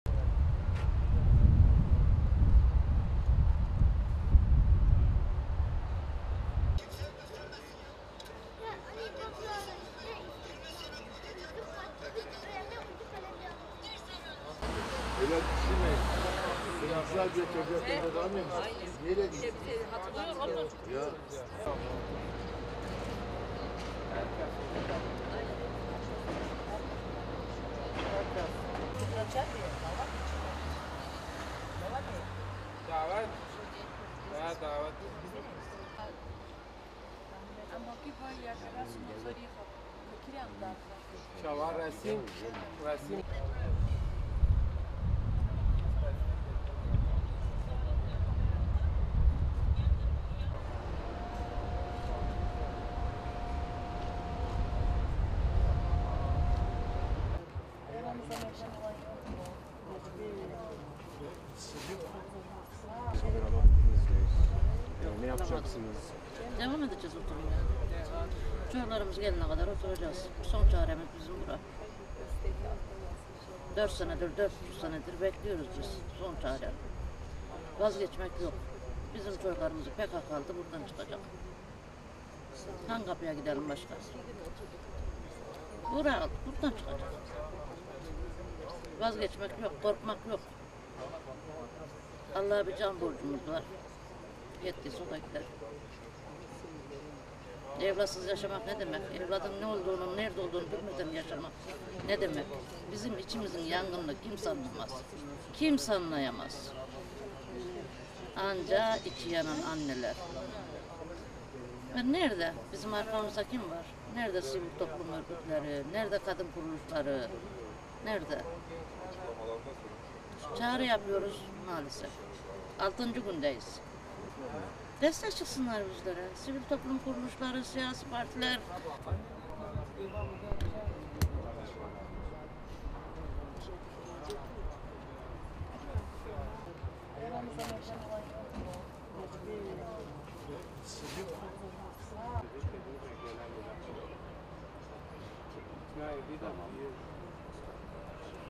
Diyarbakır'da HDP önündeki oturma eylemi 6'ncı gününde! Aile sayı 15'e yükseldi
Diyarbakır'da ailelerin HDP önündeki oturma eylemine, Ayten ve Şadin Elhaman'ın da katılmasıyla sayı 15'e yükseldi. Elhaman çifti, 2 yıl önce kaybolan oğulları Bayram (21) için oturma eylemine başladı. Ayten Elhaman, 2 yıldır oğlunun gelebileceği ümidiyle sevdiği yemekleri yapıp, kıyafetlerini yıkayıp ütülediğini söyledi. Elhaman, "Çocuğumun hiçbir siyasi bağlantısı yoktu. Bizi izliyorsan haber ver. Yaşayıp, yaşamadığını bilmiyoruz. 2 yıldır oğlumdan gelecek bir haber bekliyorum. Bir ümitle sevdiği yemekleri yapıyorum. Kıyafetlerini yıkayıp ütülüyorum" dedi.